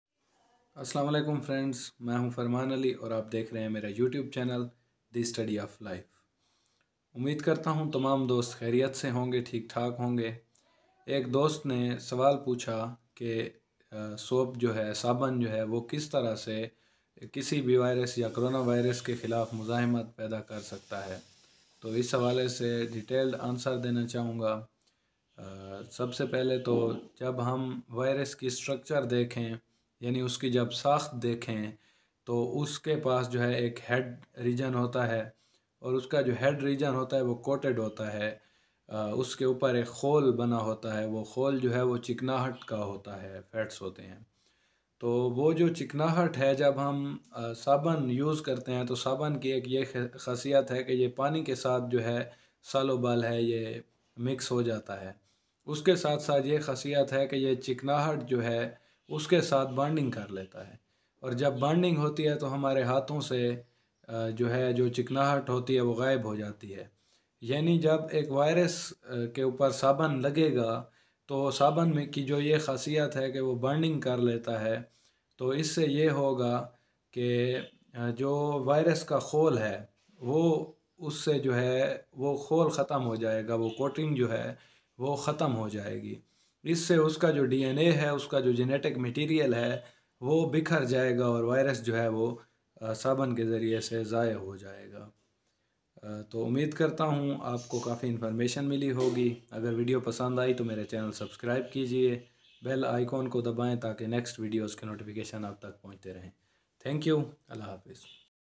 Kia Saban Se Hath Dhona Virus Se Bacha Skta Hai? Bacha Skta Hai to Kaisy? | Janiye Virus k Mutaliq
Instagram.com/Fomibaloch